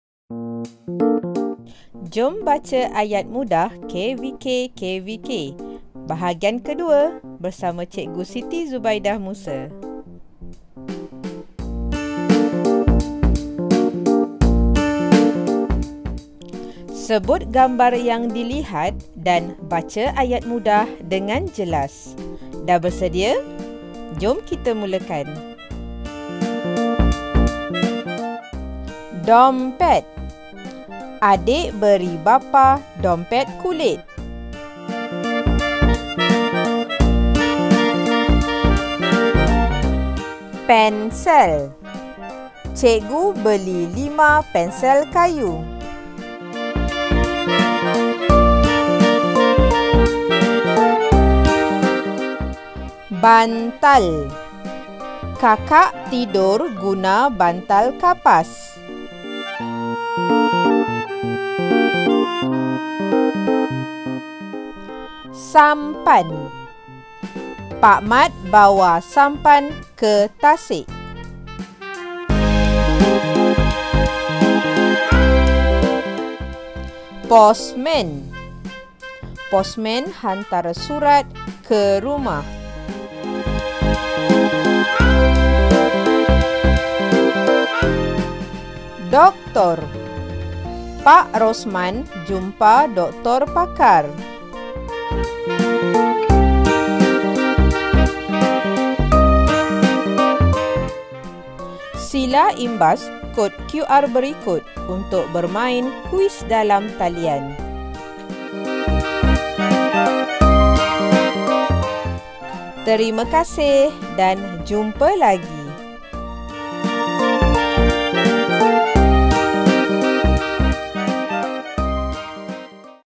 Ayat Mudah KVKKVK 2 - Sebut dan Baca
Video ini dibina untuk menjadi panduan bacaan dalam Modul Bina Ayat yang boleh didapati di pautan berikut: mylink.la/bycikguctzubed

Guru-guru juga boleh menggunakan video ini sebagai salah satu bahan bantu mengajar untuk menjalankan aktiviti imlak dalam kelas online.

Terdapat kuiz dalam talian yang boleh dimainkan oleh murid di akhir video ini.